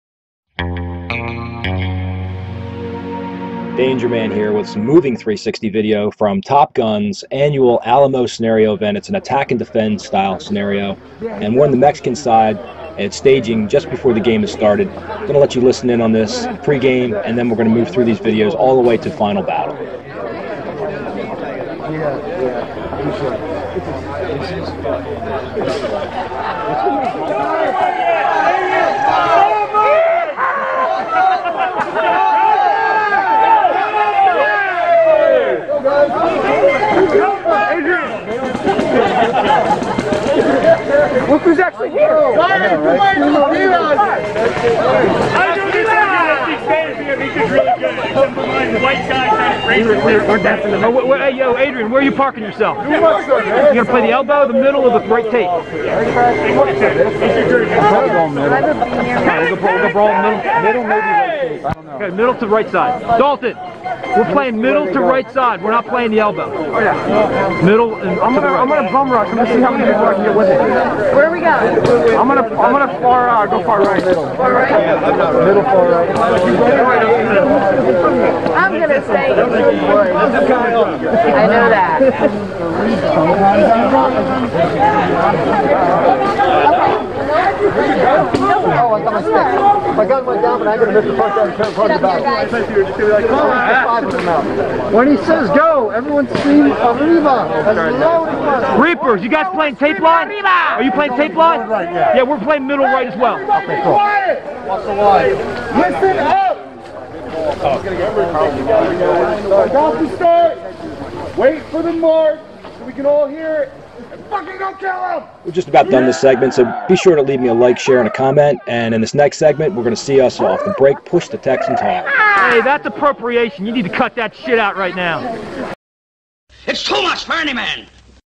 TopGun Alamo Scenario Woodsball Paintball 2018 360 VR Video Pre Game Part 1
Watch TopGun's Alamo Opening and Closing battles from beginning to end starting here.

We start at the Mexican staging area for pre game.